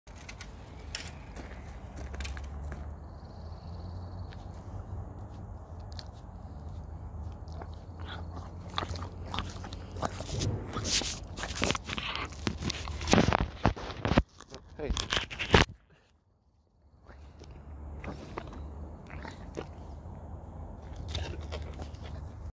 Consumed by horse (evil vertical video)
My mans took a bite of my phone